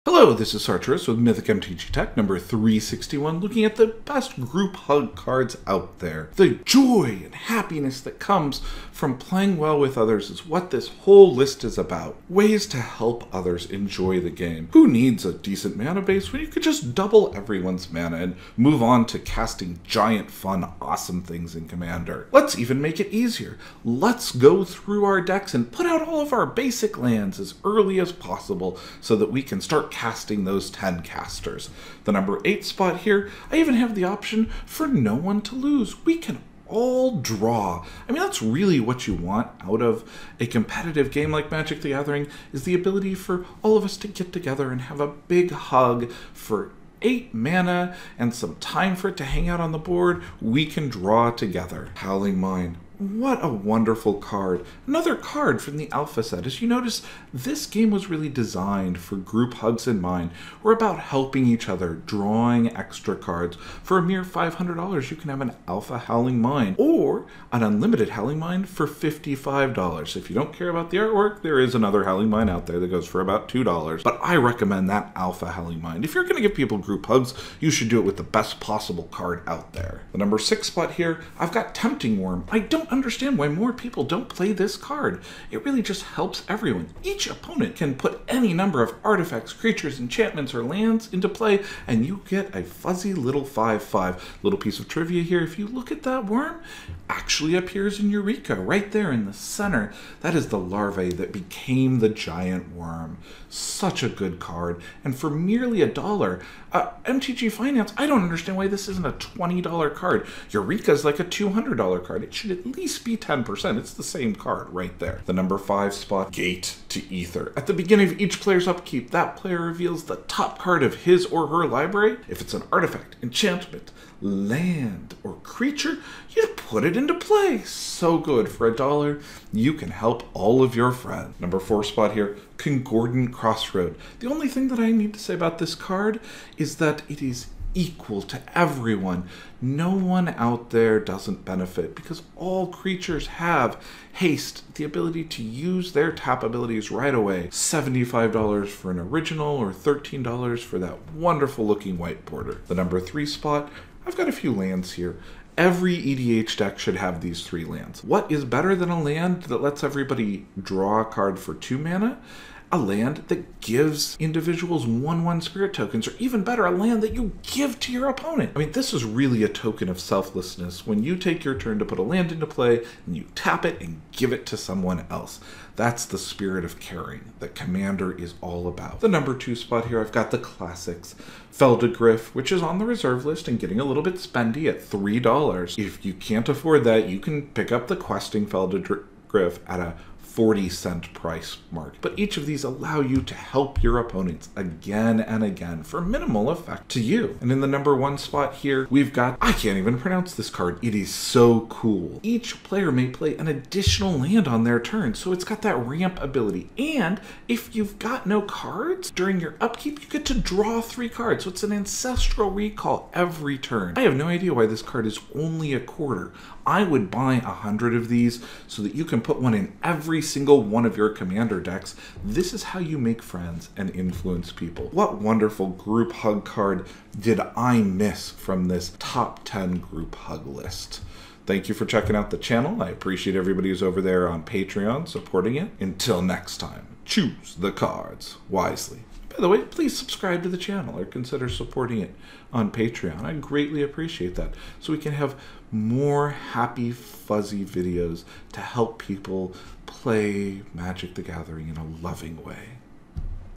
Group Hug Top 10 Commander Cards - Mythic MTG Tech # 361
This video is all about how to make friends and influence people is the social and fun format of Commander.